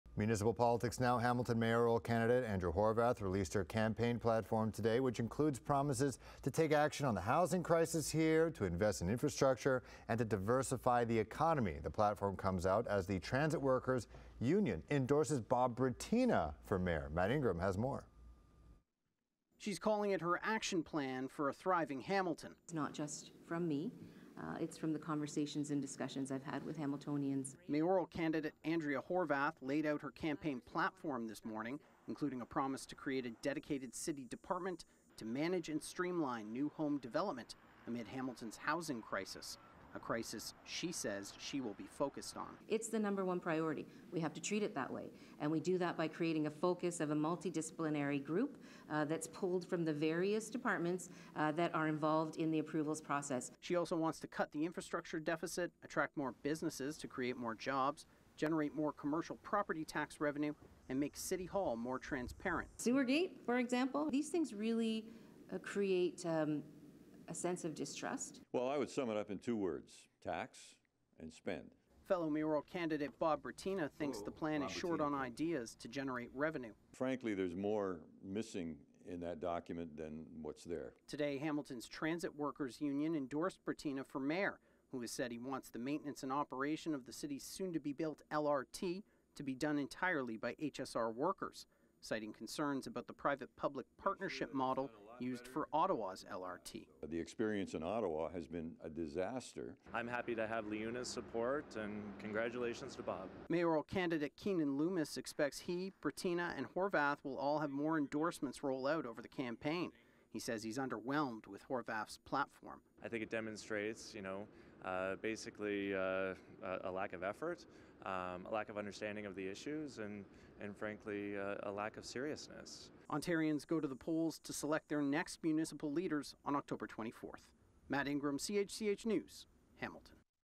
Hamilton mayoral candidate, Andrea Horwath plans to take action on housing crisis, if elected
Hamilton mayoral candidate Andrea Horwath released her campaign platform today,
which includes promises to take action on the housing crisis, invest in infrastructure, and diversify the economy.

The platform comes out as the Transit Workers Union endorses Bob Bratina for mayor who has said he wants the maintenance and operation of the city’s soon-to-be-built LRT to be done entirely by HSR workers, citing concerns about the private-public partnership model used for Ottawa’s LRT.

Horwath wants to cut the infrastructure deficit, attract more businesses to create more jobs, generate more commercial property tax revenue, and make city hall more transparent.

Mayoral candidate Keanin Loomis expects he, Bratina and Horwath will all have more endorsements roll out over the campaign.

Loomis says he is underwhelmed with Horwath’s platform.

Ontarians go to the polls to select their next municipal leaders on October 24th.